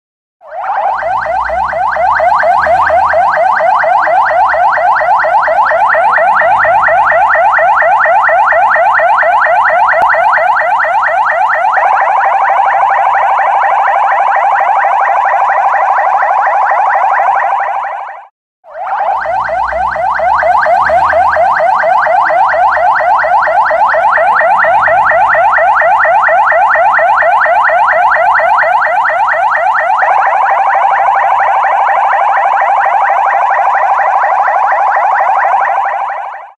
Ambulance misterius sering nyala sendiri
hal aneh mobil ambulan ini sering nyala sendiri desa malang nengah purwakarta